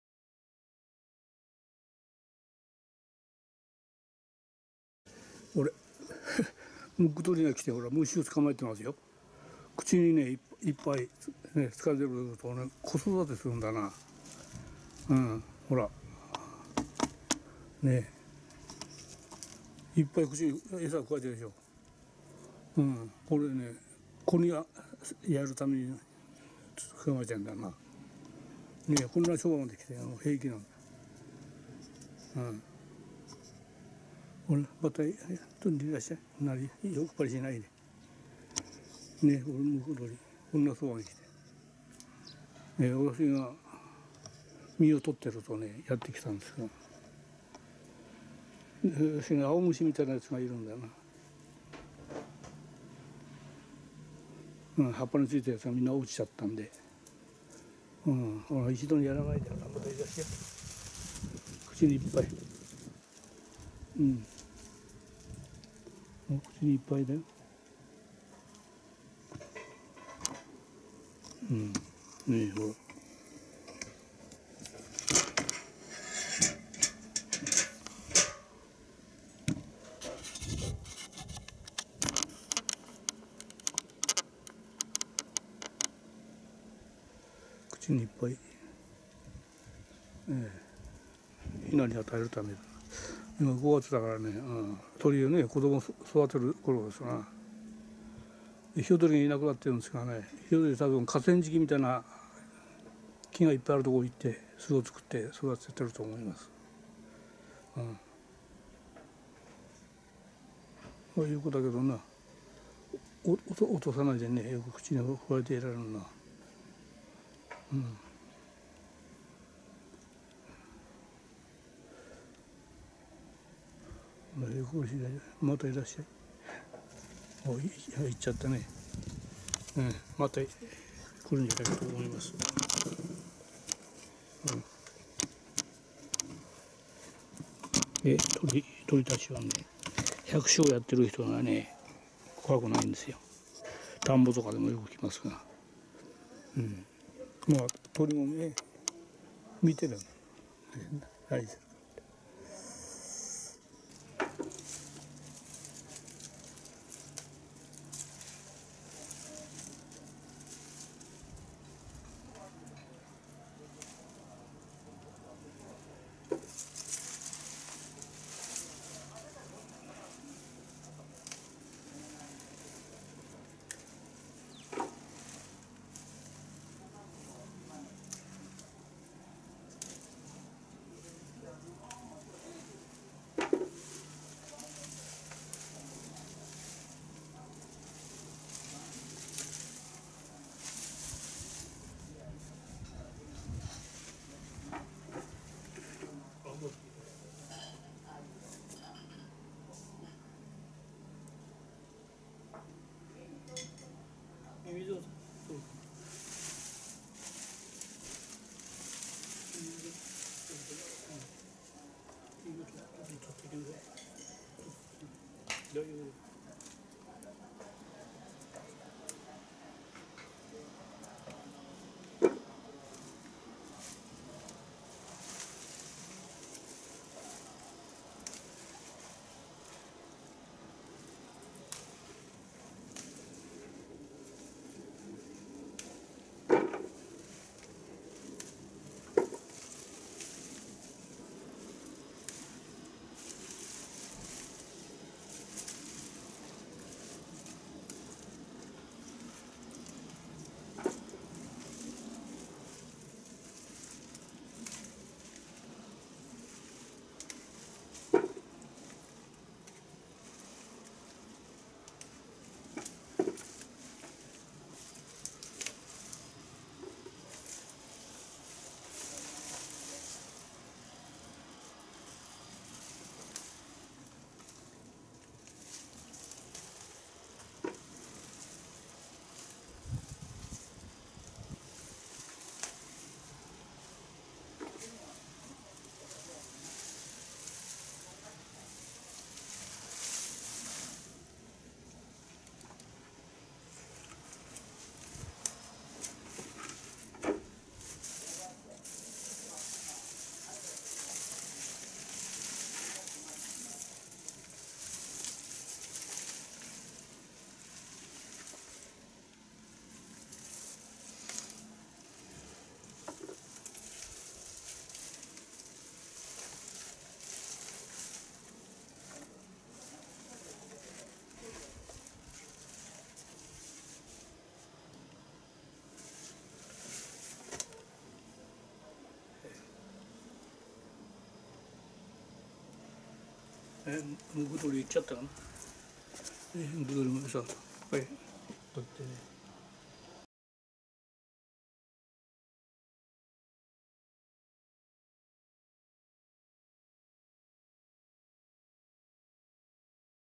令和５年５月１１日隠居宅にムクドリがやってきた
令和５年５月１１日庭の畑でスナップエンドウを片づけ、さいごの収穫をしているとムクドリがやってきた。スナップエンドウの菜っ葉についていた青虫が落ちたので取りにやってきた。くちばしにたくさんの青虫を加えている。良く落とさないでくわえられるものだと思う。今は鳥たちの子育ての時期である。子供に食べさすためである。